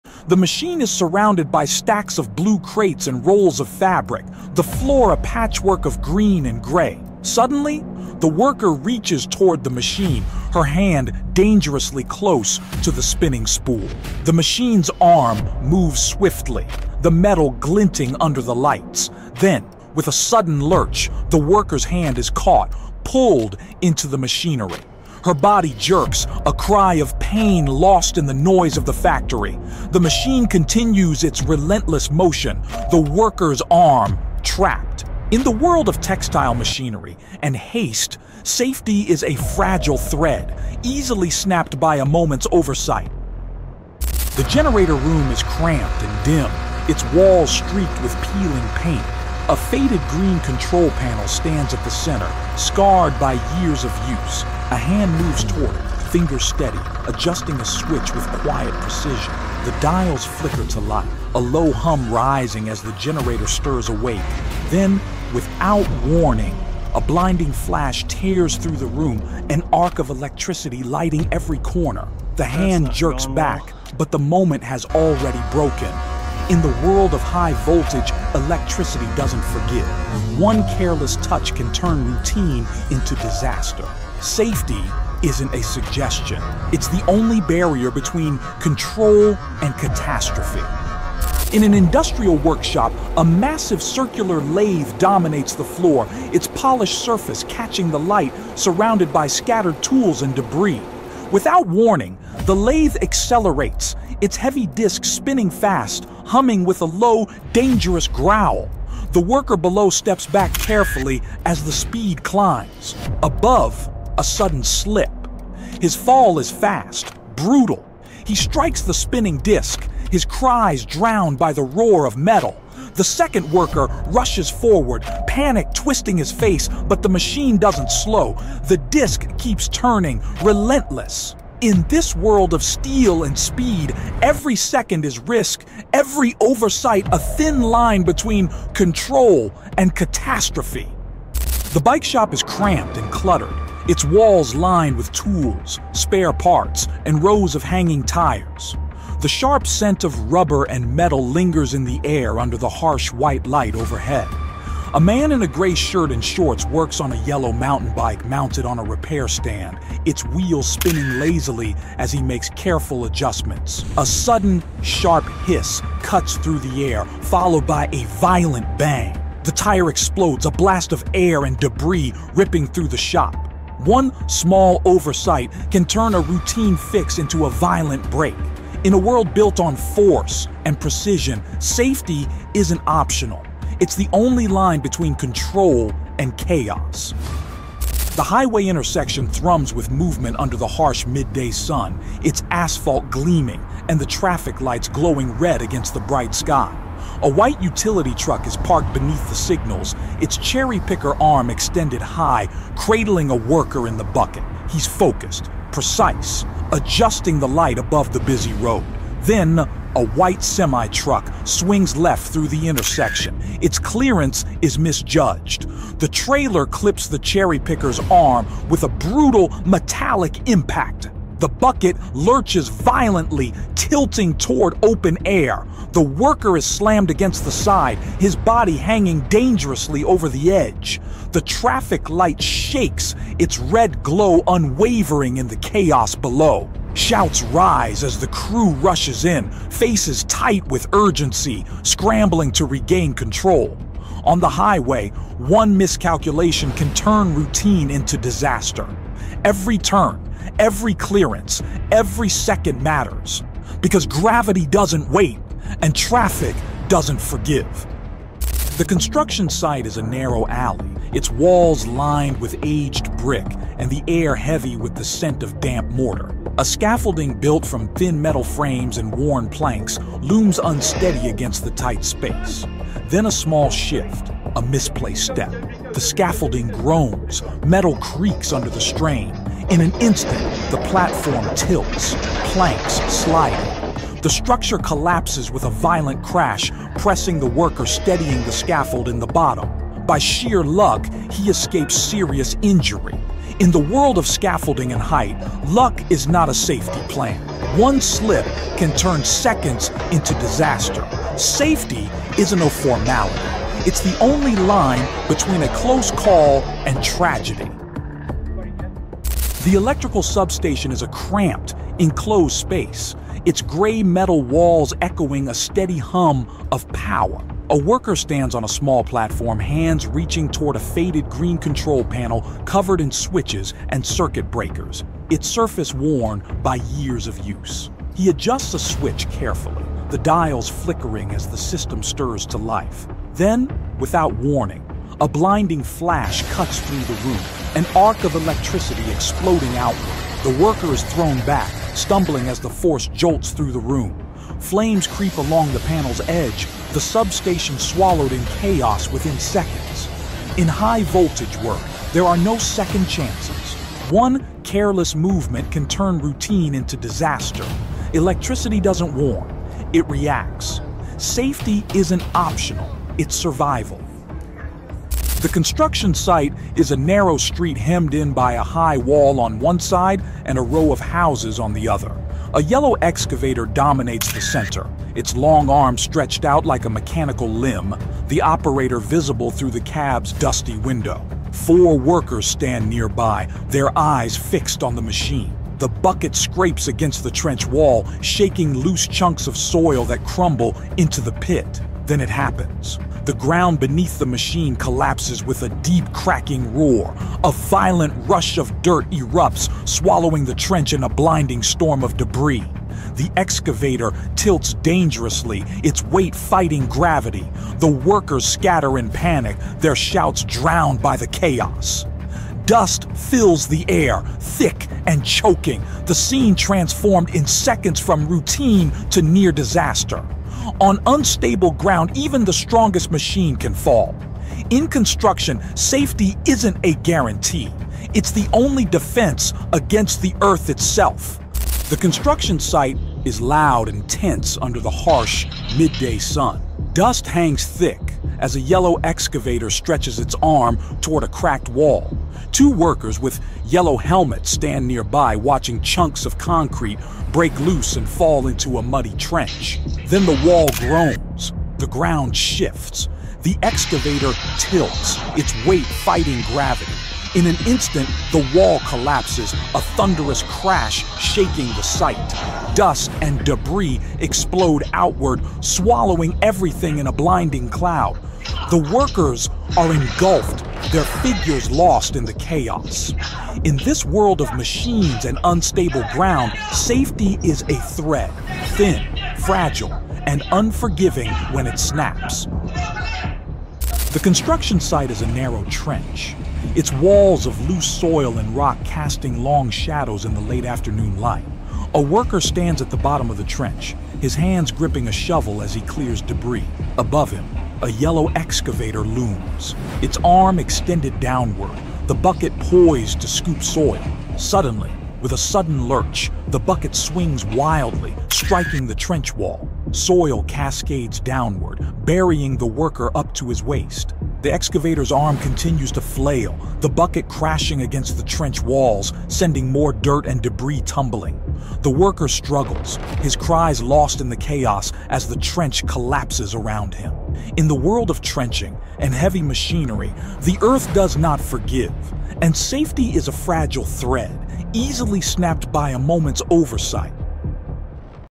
She Got Too Close… Then This Happened
Gravity doesn't negotiate. Momentum doesn't hesitate. In the world of heavy industry, the laws of physics are the ultimate judge, jury, and executioner. This video is a brutal exhibition of their verdicts.

Watch as thousands of pounds of steel, once suspended, obey the pull of the earth with devastating force. See how a sudden stop transforms secured cargo into an unguided missile. Witness the catastrophic transfer of energy as machinery fails, structures collapse, and human error leads to an immediate, unforgiving sentence.

These aren't "accidents" in the traditional sense; they are the predictable, terrifying outcomes of physics in action. This is a crucial lesson in the forces we work with every day, and a stark reminder that respecting them is the only path to survival.

If you are or represent the copyright owner of materials used in this video and have a problem with the use of said material, please send us an email, , and we can sort it out.